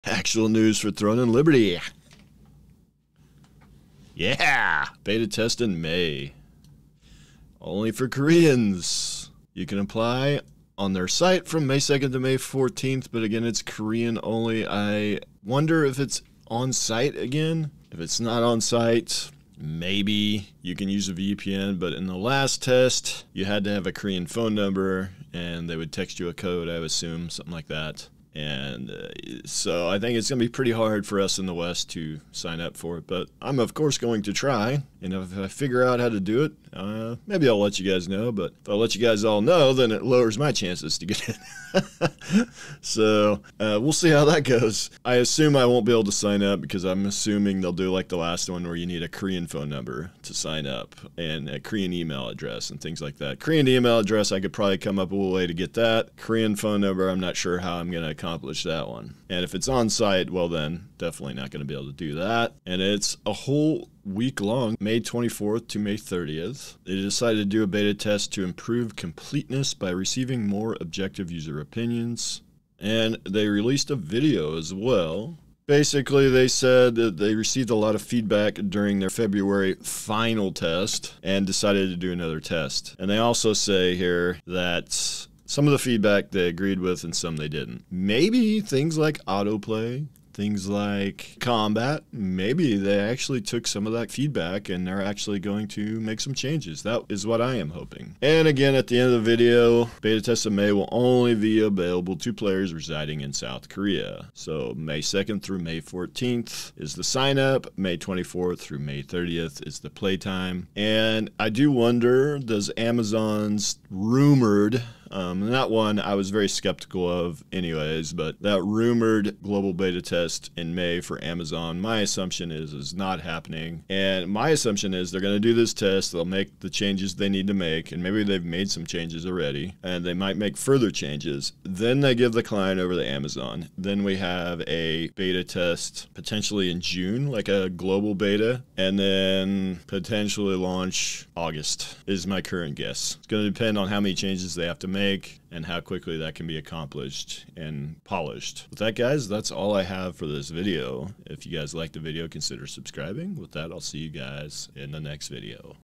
Finally... REAL OFFICIAL TL News!!! | Throne and Liberty
REAL NEWS.  Another Korean Closed Beta Test is on the way.  Sign-ups happening in early May, and the test is in late May.  They apparently heard the feedback and want to test again.  My guess is the May Global Test is not happening now, maybe in June??
✅WEBSITE+EVERYTHING KNOWN VIDEO✅
All the "EVERYTHING KNOWN" documents are now converted to the website!
🌟SOCIALS🌟
🥇 NEED A GUILD? 🥇